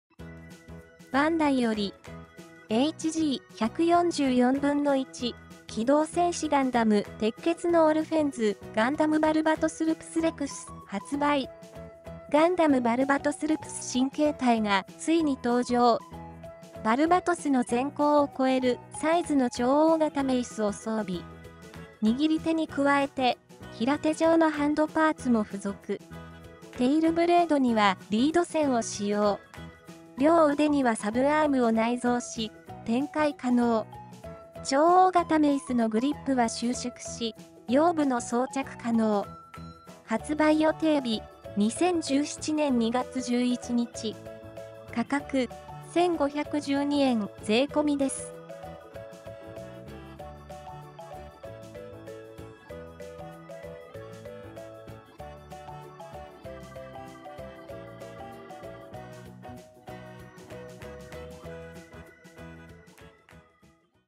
HG 1/144 機動戦士ガンダム 鉄血のオルフェンズ ガンダムバルバトスルプスレクス 発売!!
■HG 機動戦士ガンダム 鉄血のオルフェンズ ガンダムバルバトスルプスレクス 1/144スケール 色分け済みプラモデル（リンク先：Amazon）

ガンダム・バルバトスルプス新形態がついに登場！
バルバトスの全高を超えるサイズの超大型メイスを装備。
握り手に加えて、平手状のハンドパーツも付属。
テイルブレードにはリード線を使用。両腕にはサブアームを内蔵し、展開可能。
超大型メイスのグリップは収縮し、腰部の装着可能。